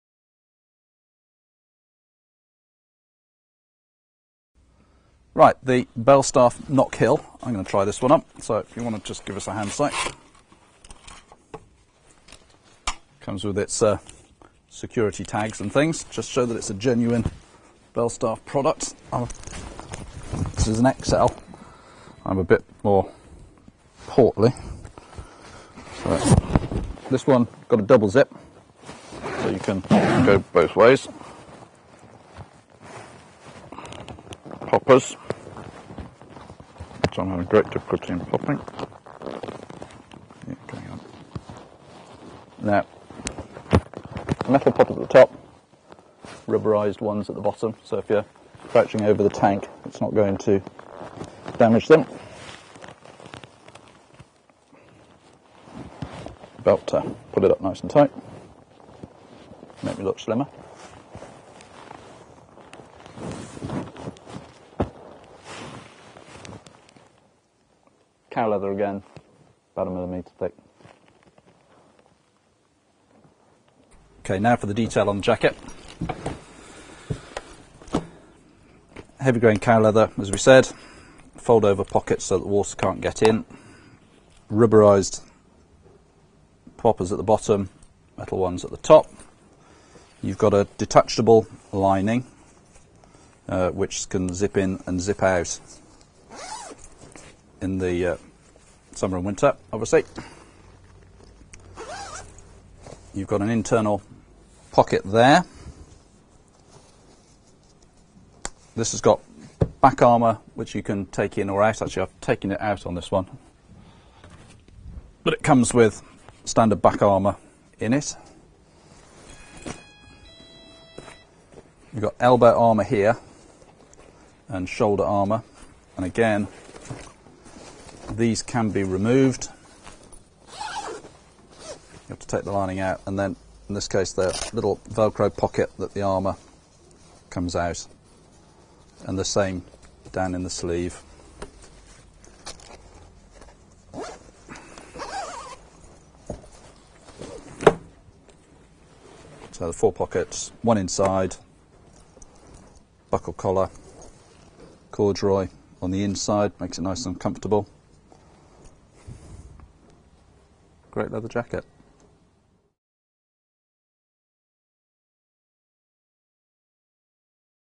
Belstaff Knockhill Trialmaster Vintage Leather Jacket from Holden Vintage & Classic
Julian Parker of Holden Vintage & Classic demonstrates the Belstaff Knockhill Trialmaster Vintage Leather Jacket.